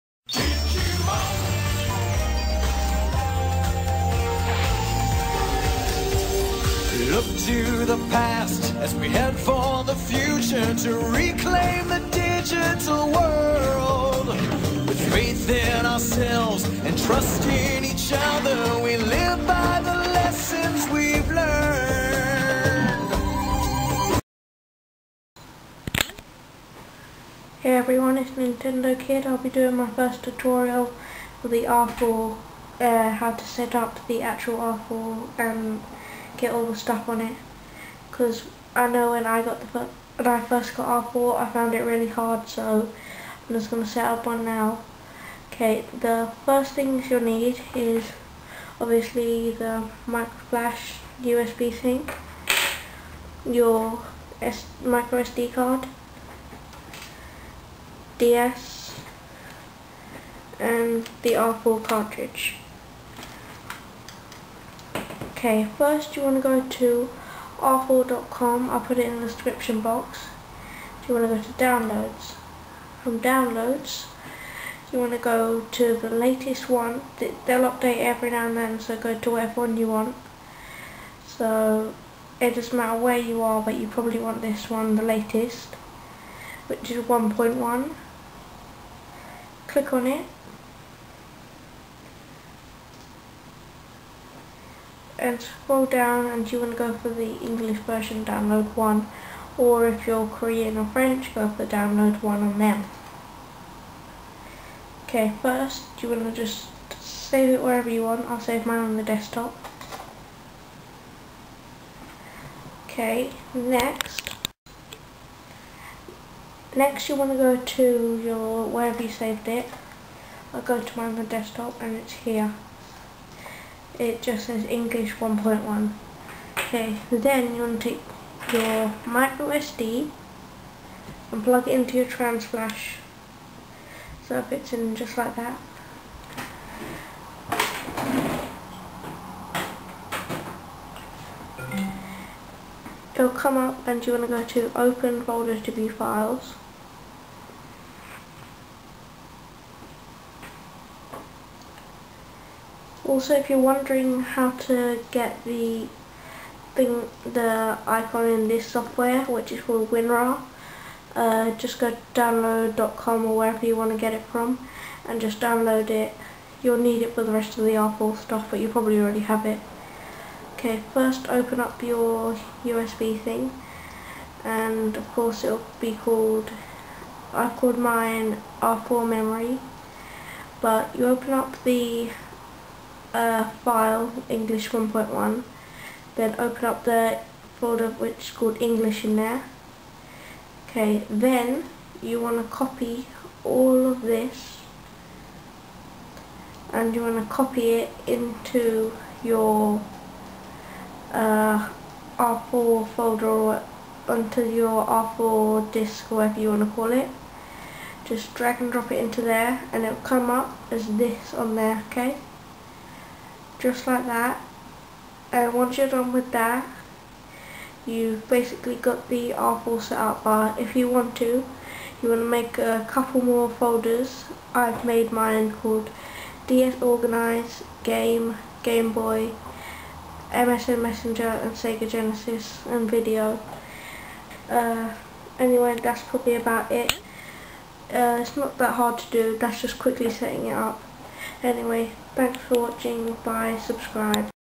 R4 tutorial Setting up r4 kernel
My first r4 tutorial on how to get it set up